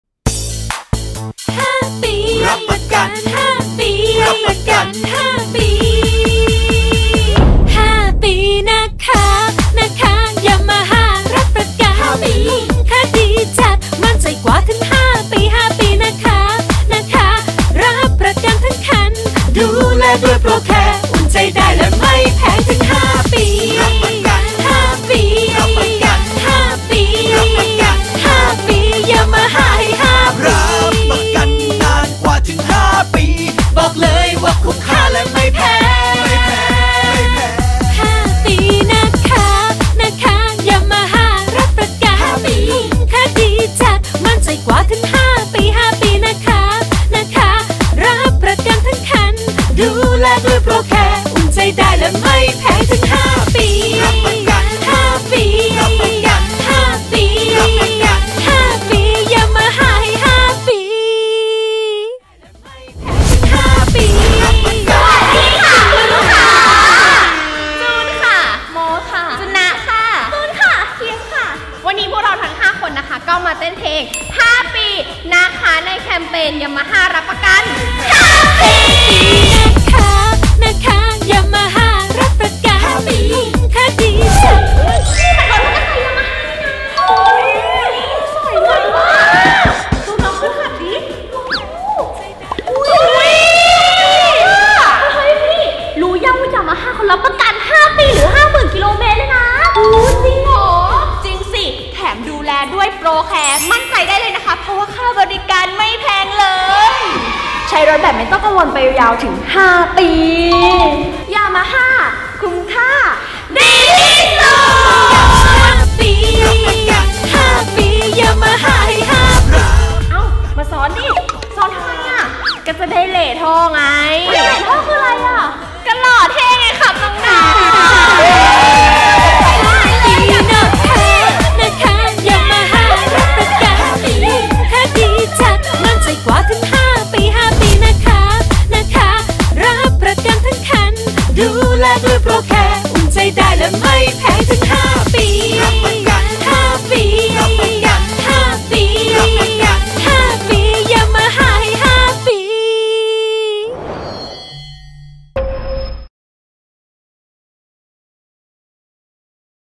เต้นให้ยับ กับความคุ้มแบบสับๆ จากยามาฮ่า ที่รับประกันทั้งคัน นานกว่าใครถึง 5 ปี หรือ 50,000 กิโลเมตร
5 ปี นะคะ นะคะ 🎵
ยามาฮ่า รับประกันทั้งคัน 5 ปี
คุ้มค่า ดีจัด มั่นใจกว่า และไม่แพง🎵

ท่าเต้นก็น่ารัก การรับประกันก็น่าเลิฟ❤️
กับยามาฮ่าที่มอบความคุ้มค่าให้มากกว่าใคร
ด้วยการรับประกันนานกว่าถึง 5 ปี หรือ 50,000 กิโลเมตร🛵
พร้อมด้วยบริการ Pro Care ที่มั่นใจได้แบบมืออาชีพ

*สำหรับทุกรุ่นที่ไม่เกิน 500 ซีซี
และเงื่อนไขเป็นไปตามที่บริษัทฯ กำหนด

ถ่ายทำช่วงเดือน มี.ค 2564